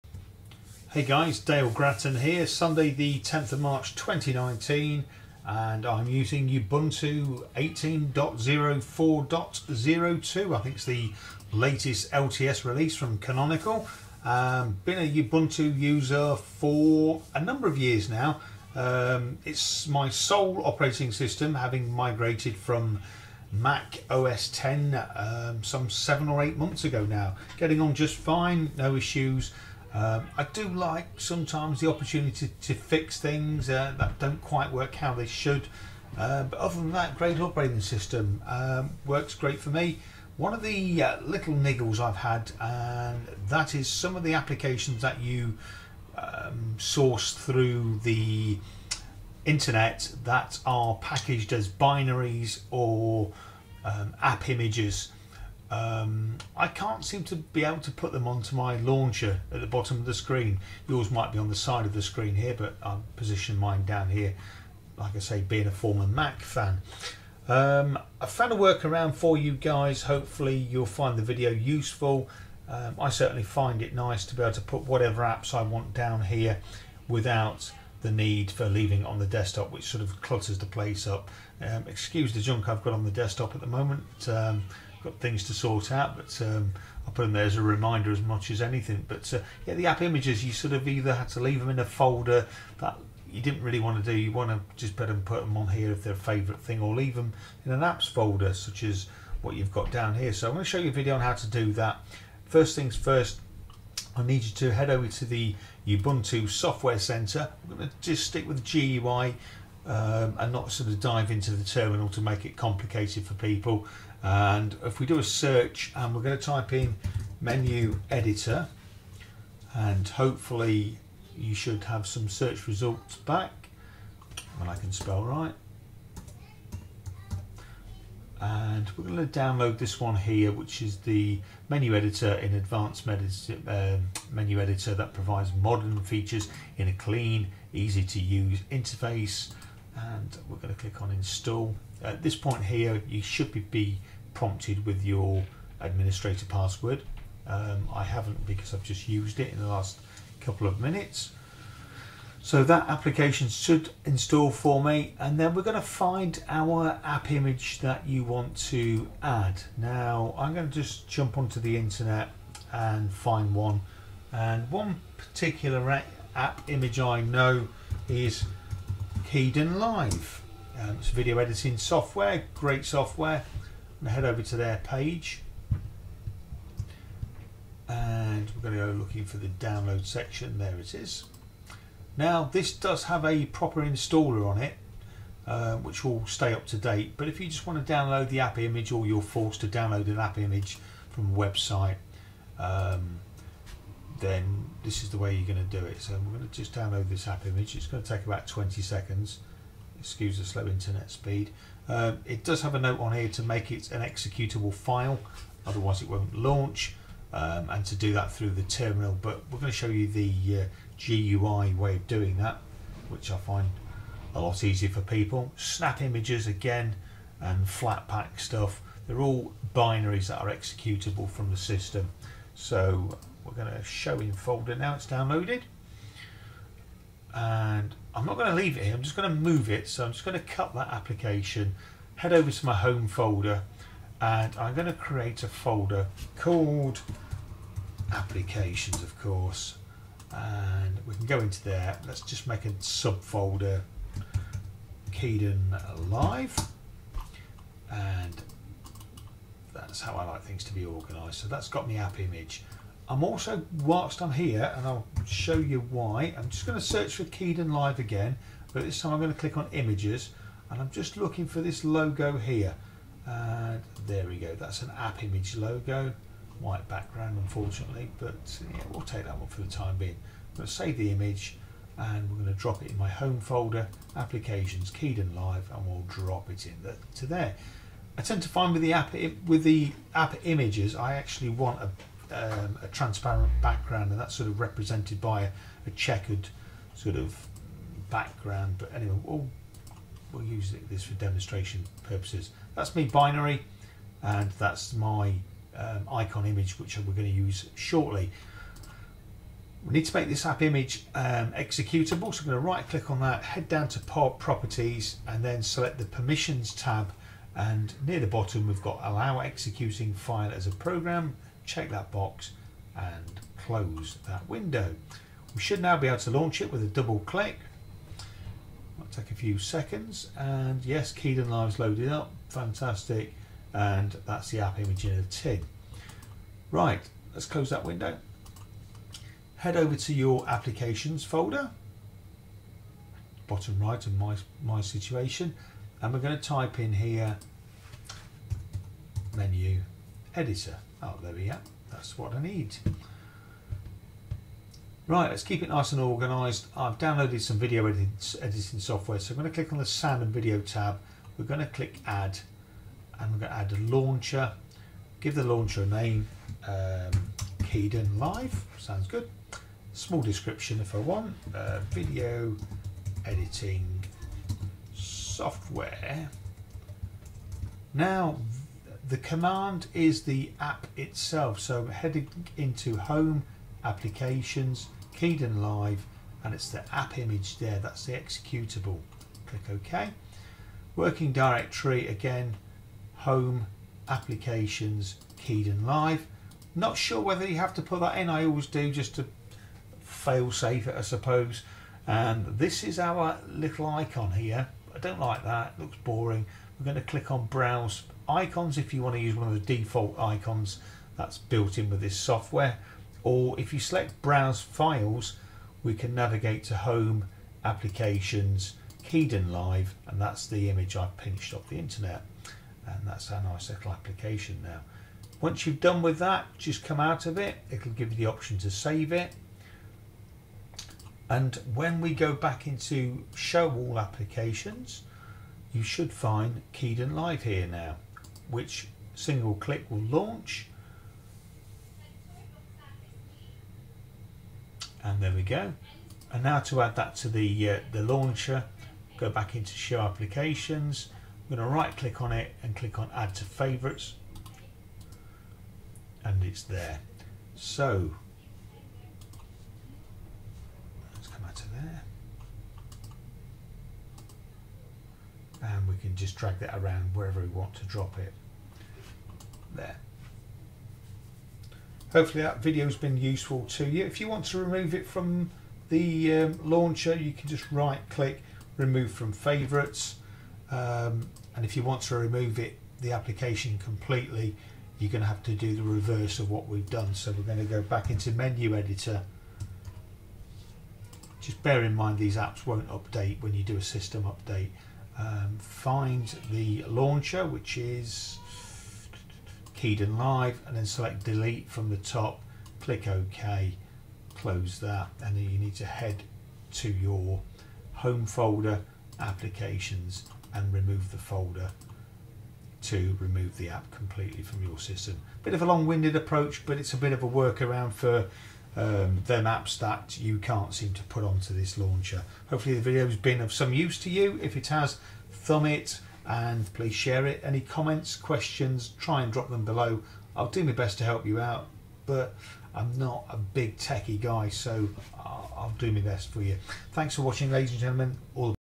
Ubuntu - adding an Appimage or Binary to the Launcher/Dock
A fairly simple way of adding an Appimage, Binary or other Executable to the Launcher / Dock.